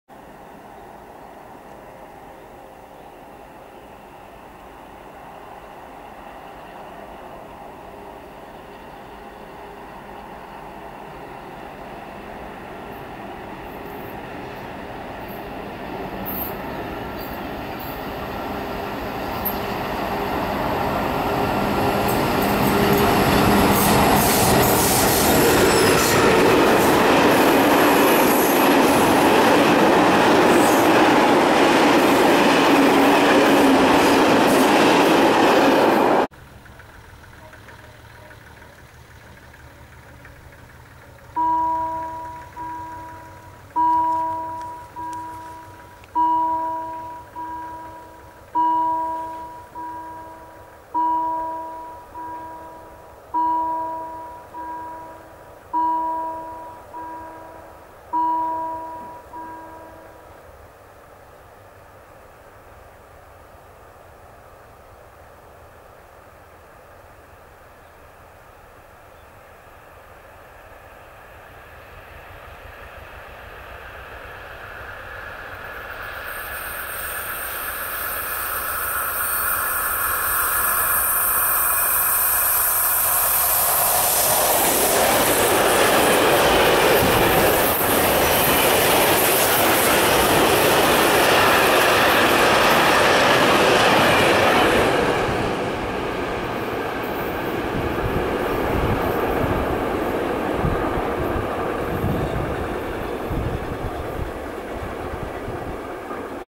Getreidezug mit 232 293-1 von EBS
Der Getreidezug aus Niedercunnersdorf wurde heute in 2 Teilen über Ebersbach (Sachs) abgefahren und in Bischofswerda vereint, da in Ebersbach (Sachs) noch Kesselwagen stehen.
Den ersten Teil des Zuges konnte ich in Neukirch (Ost) um 16:47 in Ri. Bischofswerda filmen. Den zweiten Teil habe ich dann in Taubenheim (Spree) um 19:27 gefilmt ebenfalls in Ri. Bischofswerda.